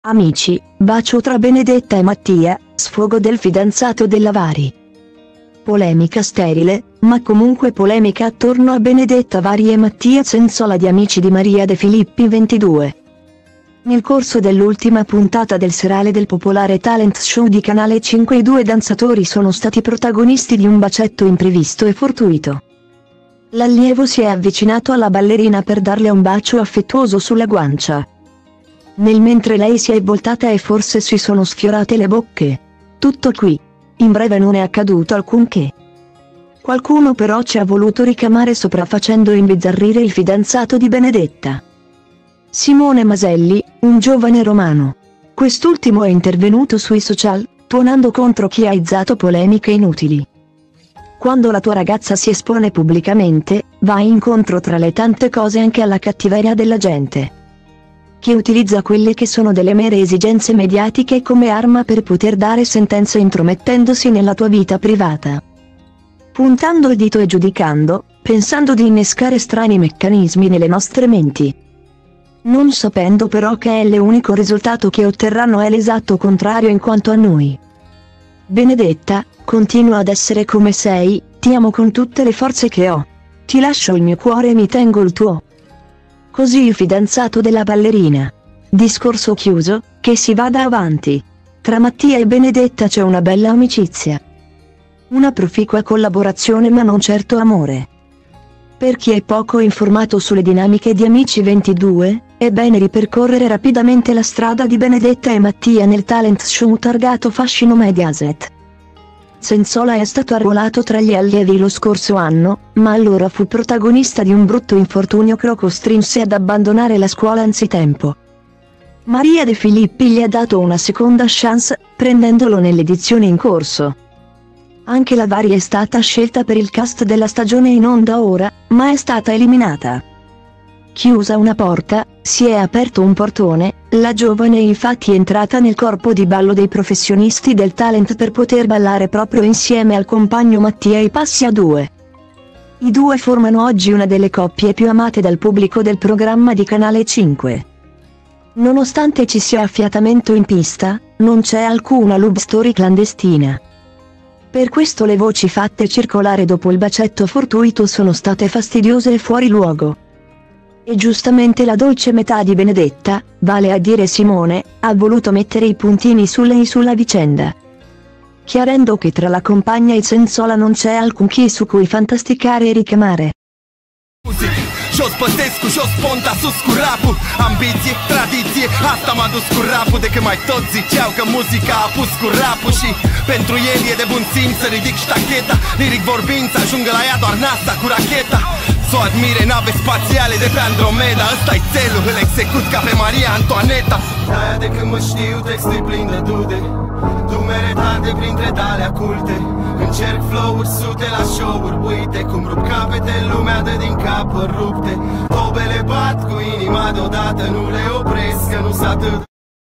Amici, bacio tra Benedetta e Mattia: sfogo del fidanzato della Vari
Polemica sterile, ma comunque polemica attorno a Benedetta Vari e Mattia Zenzola di Amici di Maria De Filippi 22.Nel corso dell’ultima puntata del Serale del popolare talent show di Canale Cinque i due danzatori sono stati protagonisti di un bacetto imprevisto e fortuito.L’allievo si è avvicinato alla ballerina per darle un bacio affettuoso sulla guancia,